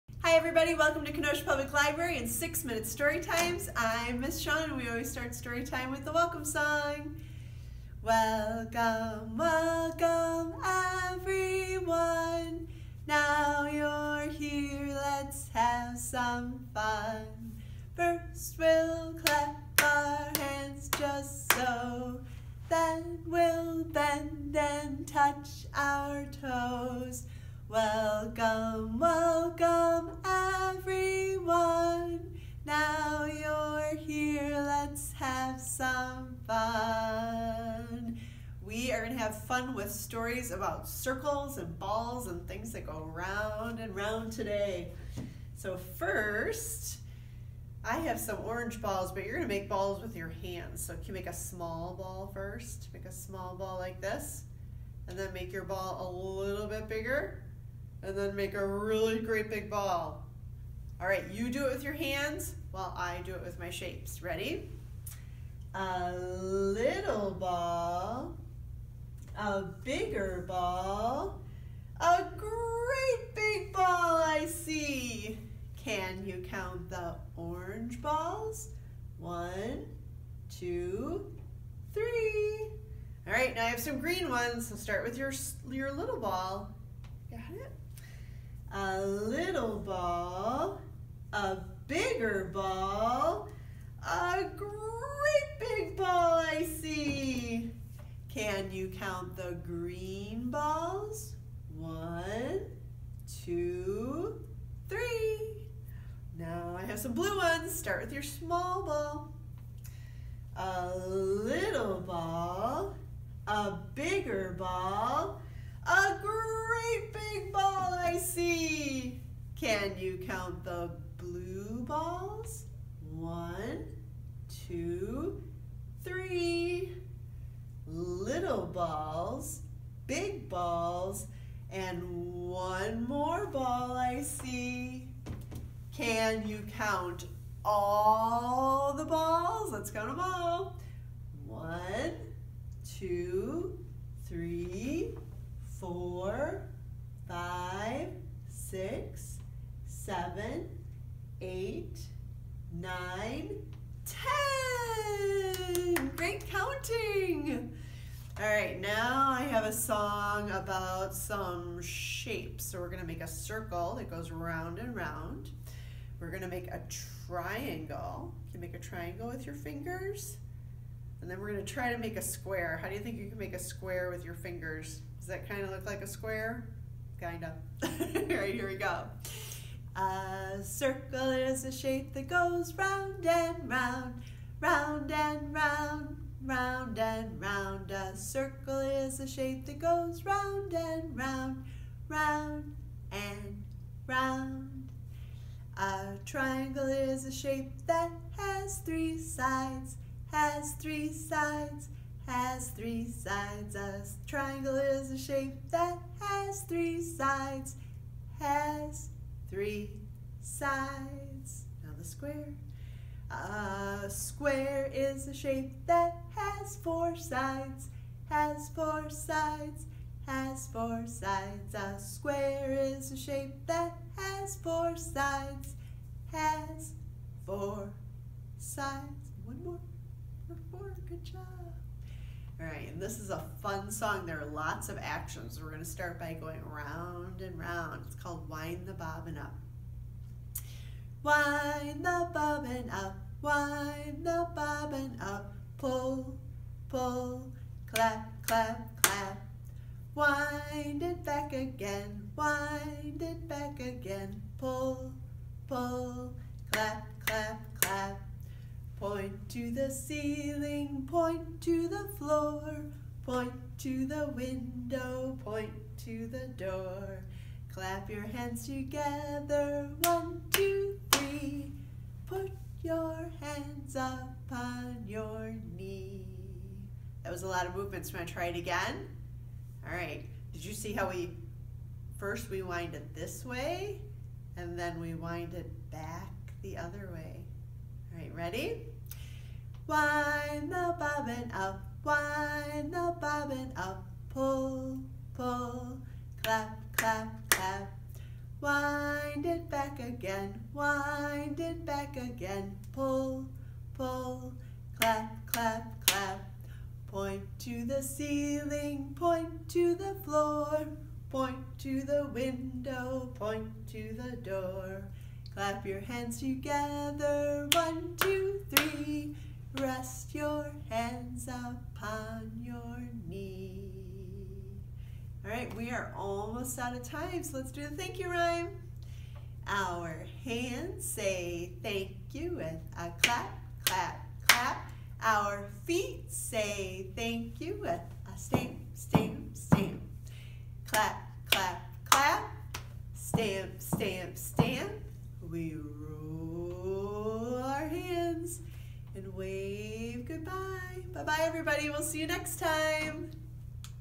Six Minute Storytime Circles
This storytime goes round and round. Join Miss Shawn for stories and songs about circles!